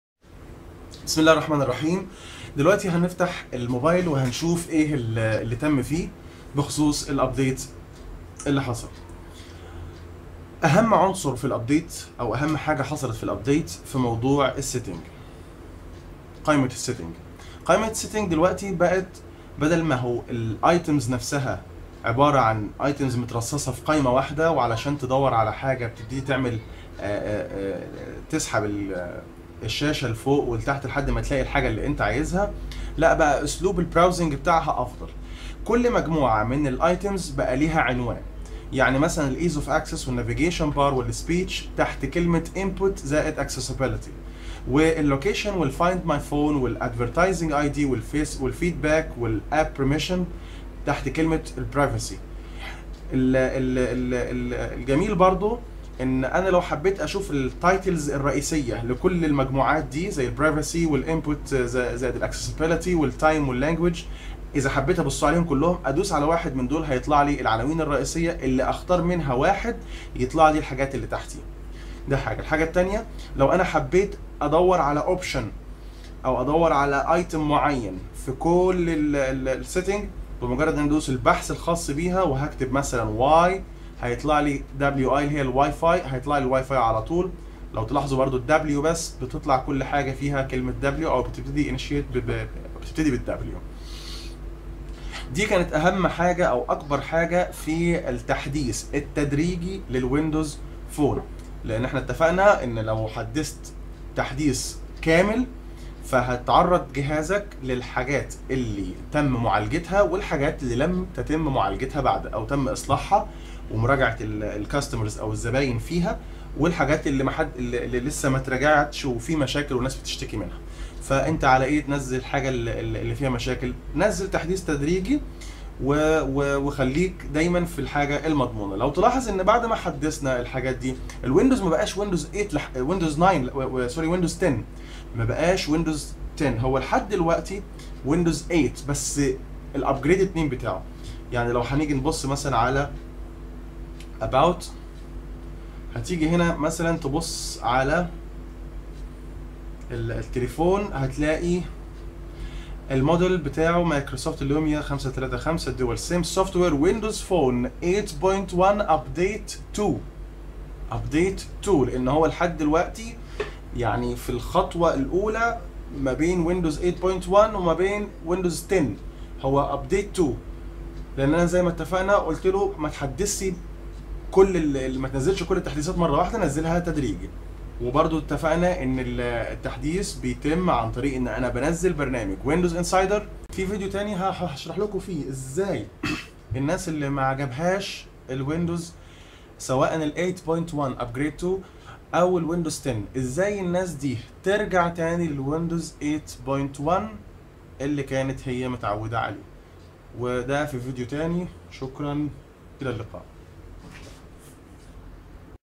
windows 8.1 update 2 --- الحلقة الثانية : الطريق للويندوز 10
الحلقة الثانية .. ضمن حلقات تطوير أجهزة الويندوز فون للوصول للويندوز
windows phone 10 preview
مراجعة ويندوز فون 10